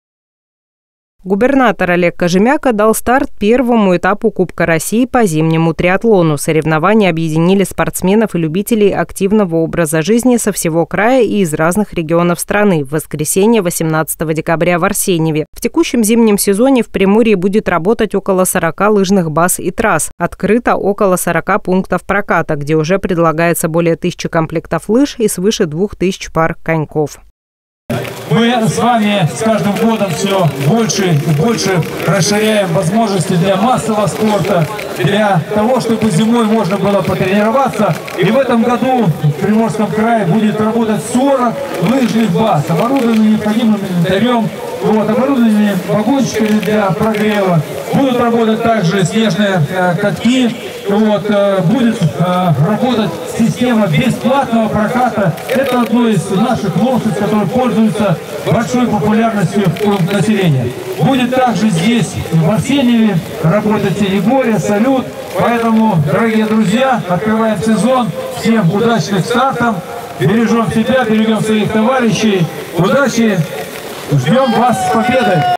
Олег Кожемяко открыл первый этап Кубка России по зимнему триатлону.
Губернатор Олег Кожемяко дал старт первому этапу Кубка России по зимнему триатлону. Соревнования объединили спортсменов и любителей активного образа жизни со всего края и из разных регионов страны в воскресенье, 18 декабря, в Арсеньеве. В текущем зимнем сезоне в Приморье будет работать около 40 лыжных баз и трасс. Открыто около 40 пунктов проката, где уже предлагается более тысячи комплектов лыж и свыше двух тысяч пар коньков.